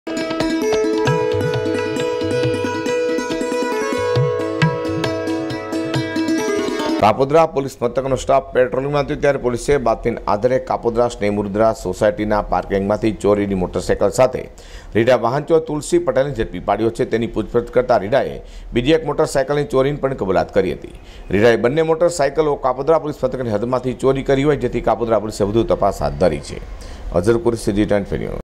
કાપોદ્રા પોલીસ રીઢા વાહન ચોરને ઝડપી પાડ્યો | G 24 NEWS
રીઢા વાહન ચોરને ઝડપી પોલીસે બે ગુનાના ઉકેલ્યા ભેદ